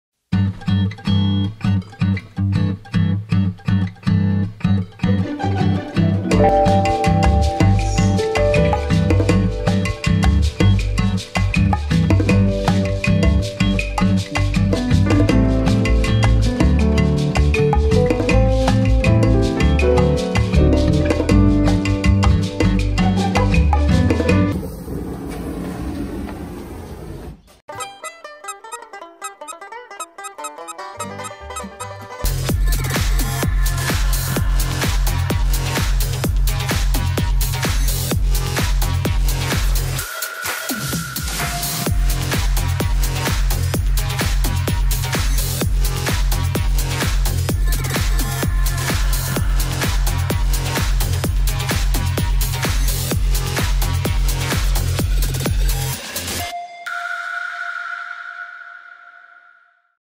SOCIAL DISTANCING TECHNIQUES
Super effective ways to take out your bin in 2020